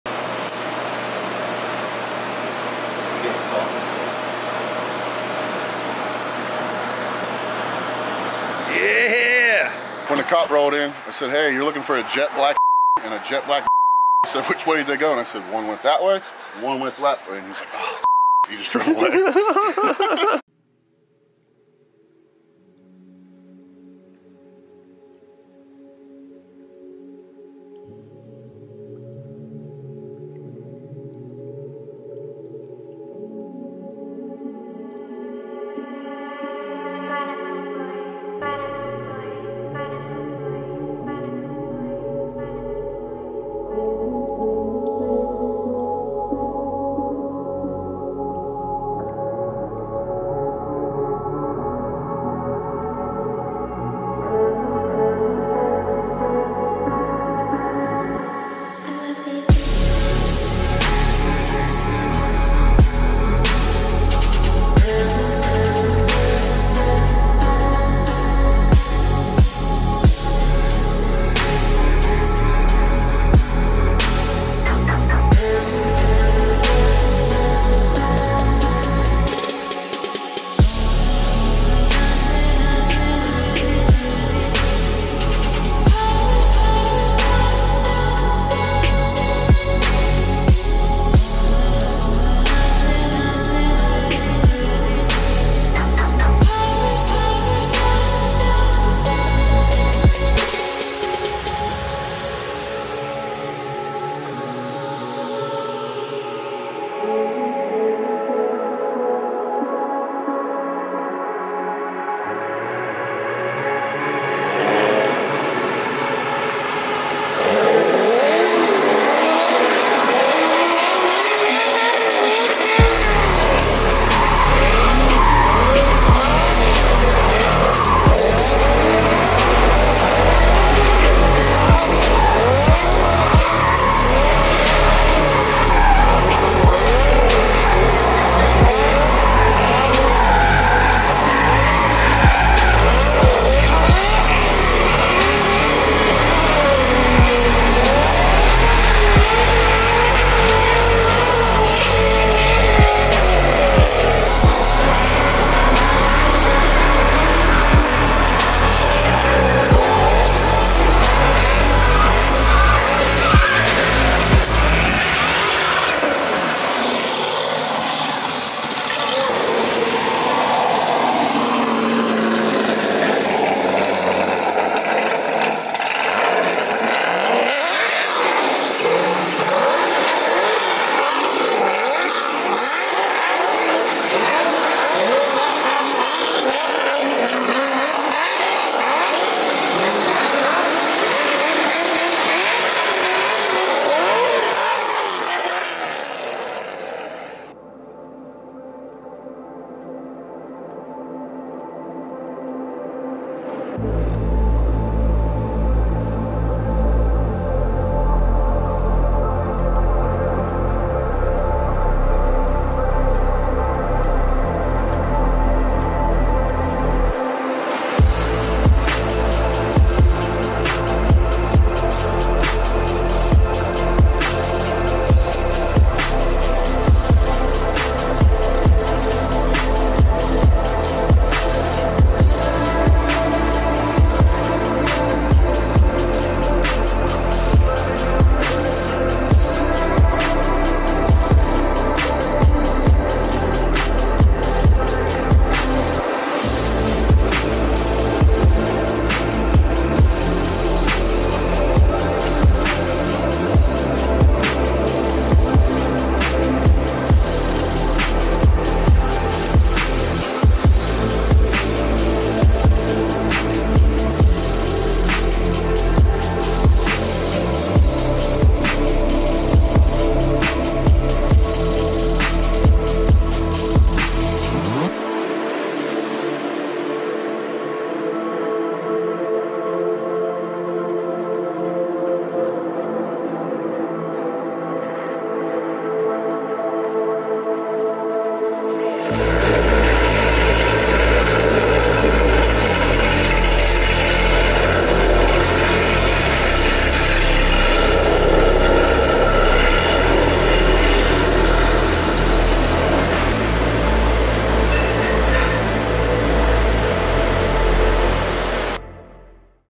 X-mas Rollers 2k23
Last year 2022 we held a small Christmas car meet. We wanted to do it again so we did. The weather beat most of us but some of us were able to squeeze it in!